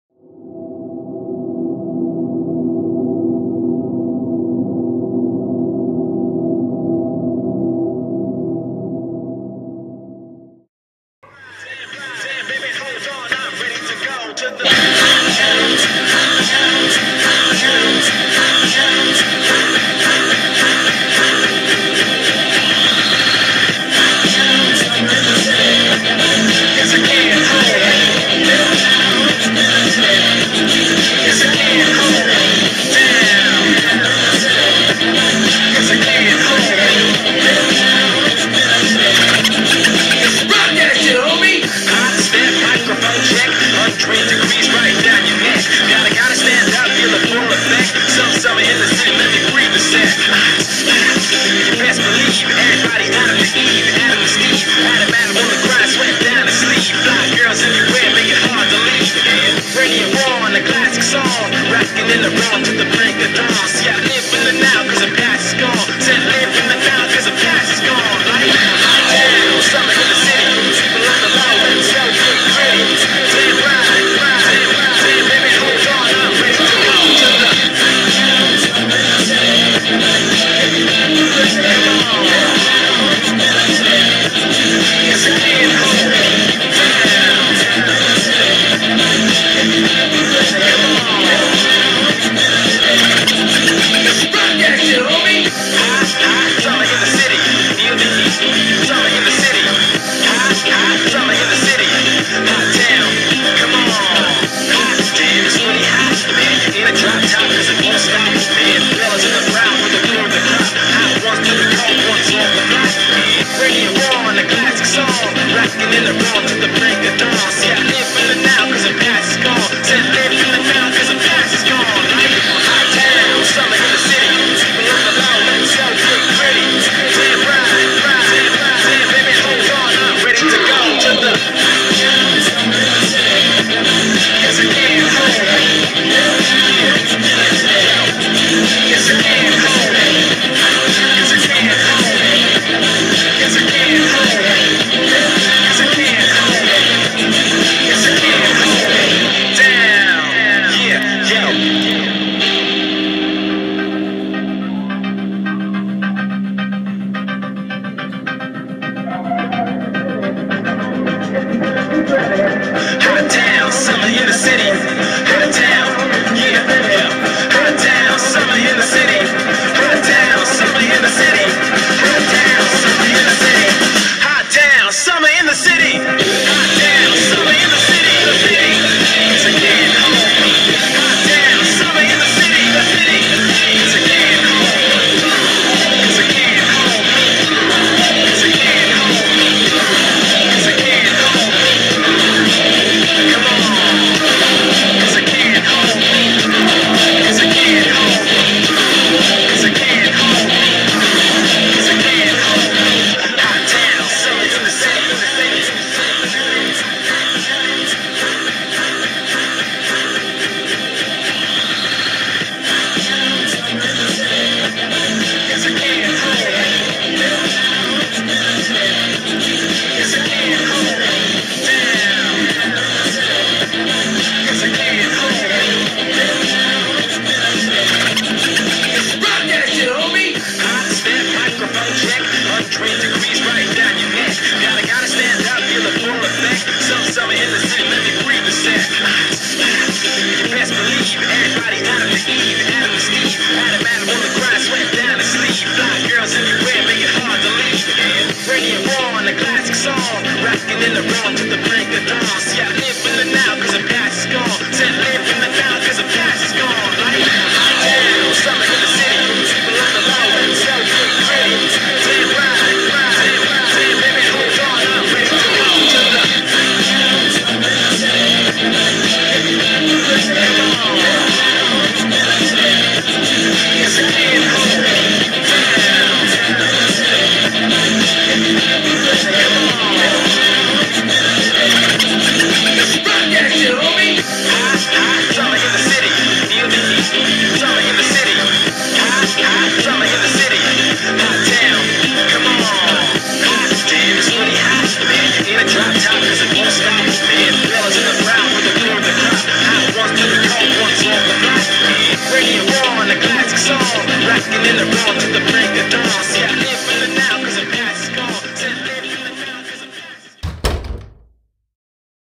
JDay Snowday A250 Lap 1 Liquid Image Summit Goggles
This is lap one of round one from A250class in Greenfield, MA 2.4.12, special thanks to Liquid Image for making this video possible!